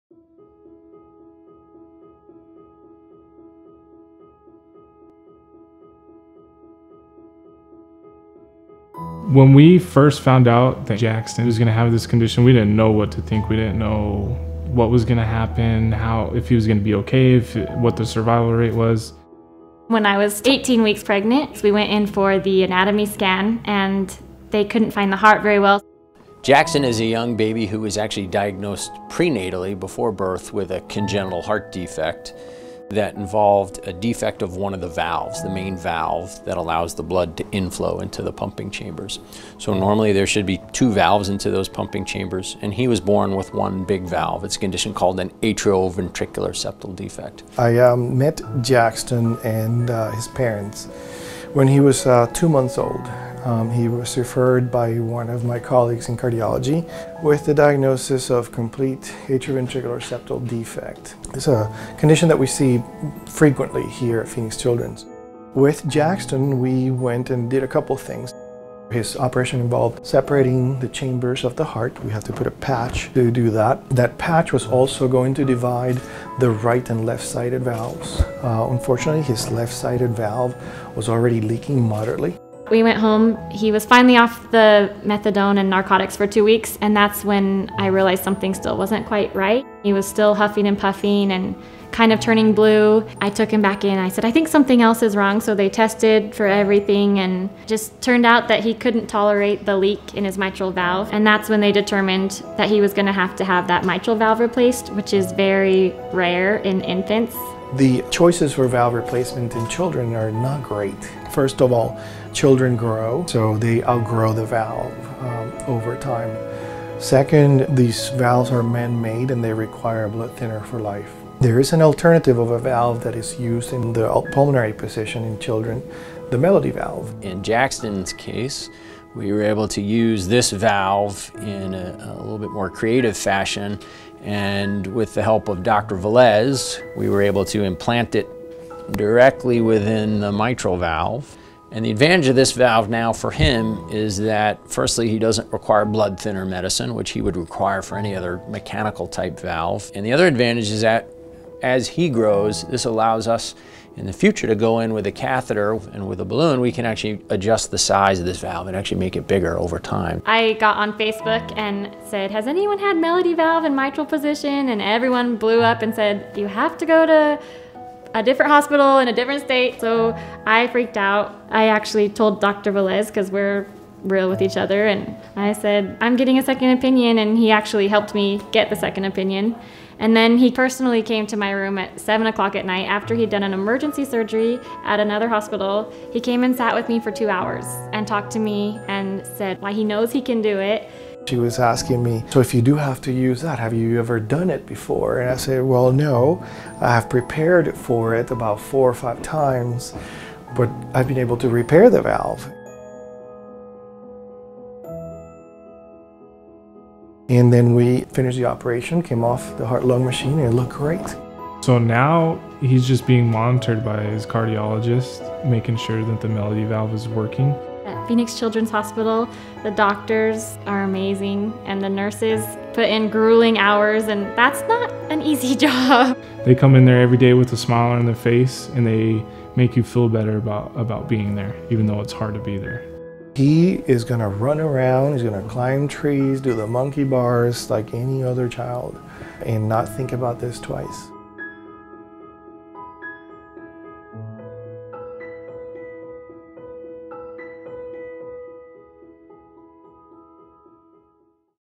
Melody Valve Heart Surgery: Jaxton's Story
Even though Heart Month has ended, we have one more story to share. It's none other than Jaxton! He is one tough little guy. Jaxton had heart surgery at 7-months old and he’s the first case in Arizona of a melody valve implantation in the mitral position (rather than the pulmonary position). Watch Jaxton's amazing story!